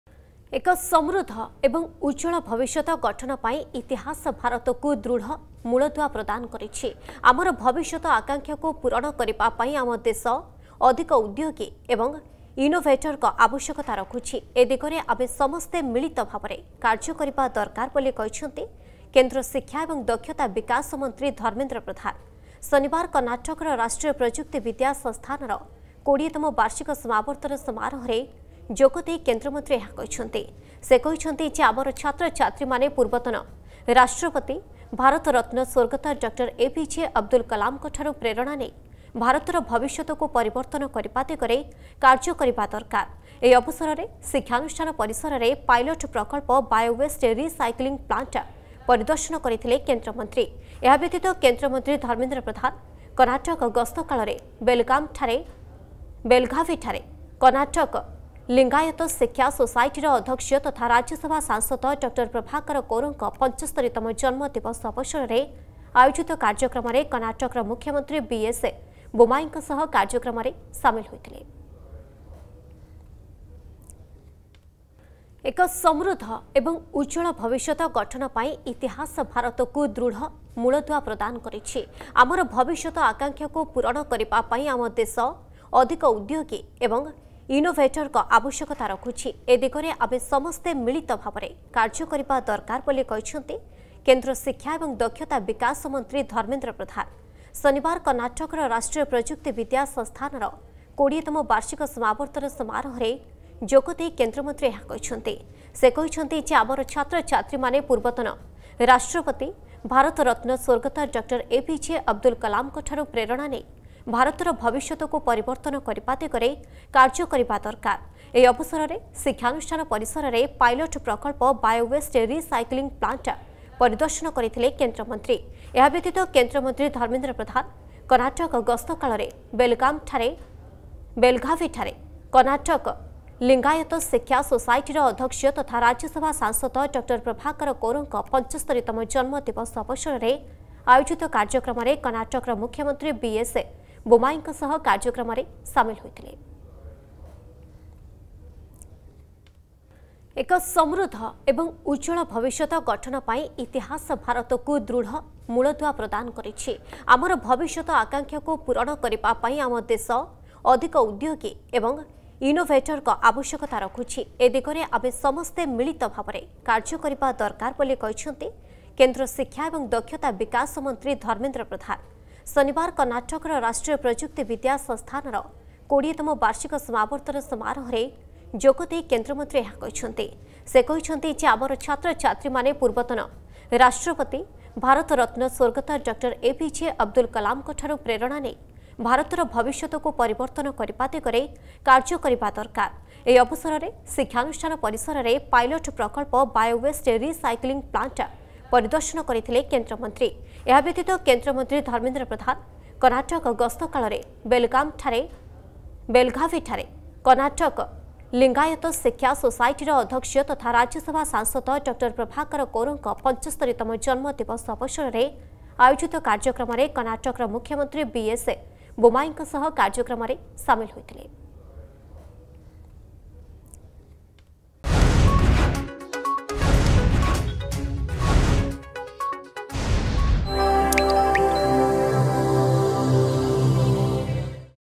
ରାଷ୍ଟ୍ରୀୟ ପ୍ରଯୁକ୍ତିବିଦ୍ୟା ସଂସ୍ଥାନର ୨୦ତମ ବାର୍ଷିକ ସମାବର୍ତ୍ତନ ସମାରୋହରେ ଯୋଗଦେଇଛନ୍ତି ଧର୍ମେନ୍ଦ୍ର ପ୍ରଧାନ
କର୍ଣ୍ଣାଟକର ରାଷ୍ଟ୍ରୀୟ ପ୍ରଯୁକ୍ତିବିଦ୍ୟା ସଂସ୍ଥାନର ସମାବର୍ତ୍ତନ ସମାରୋହରେ କେନ୍ଦ୍ରମନ୍ତ୍ରୀ ଧର୍ମେନ୍ଦ୍ର ପ୍ରଧାନ। କହିଲେ, ଦେଶବାସୀଙ୍କ ଭବିଷ୍ୟତ ଆକାଂକ୍ଷାକୁ ପୂରଣ କରିବା ଆମର ଉଦ୍ଦେଶ୍ୟ...।